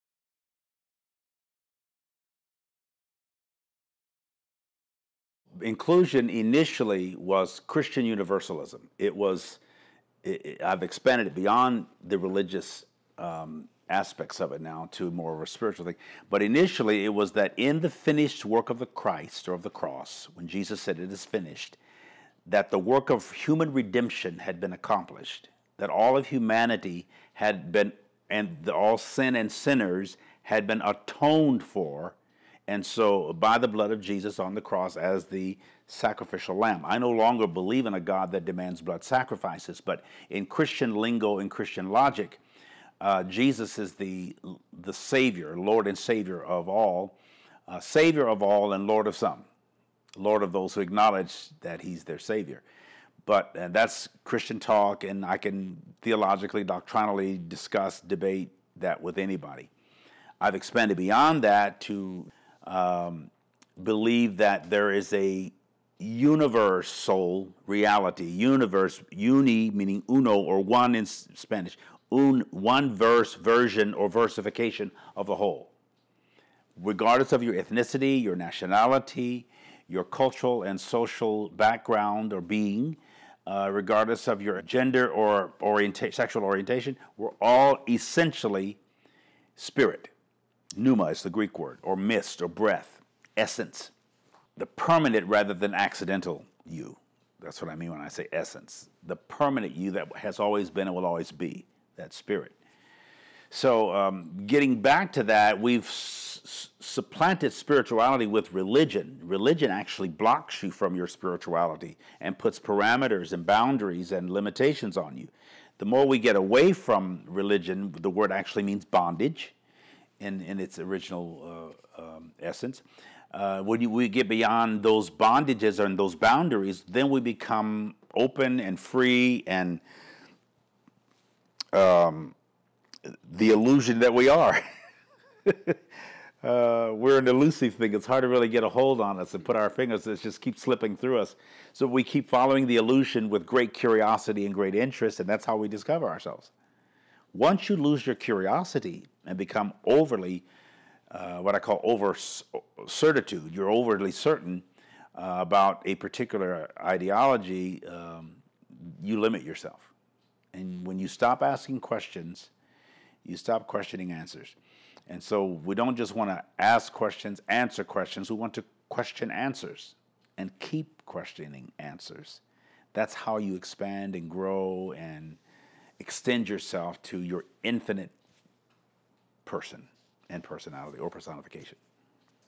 Carlton Pearson on Inclusion
Another clip from an interview with Bishop Carlton Pearson on Inclusion.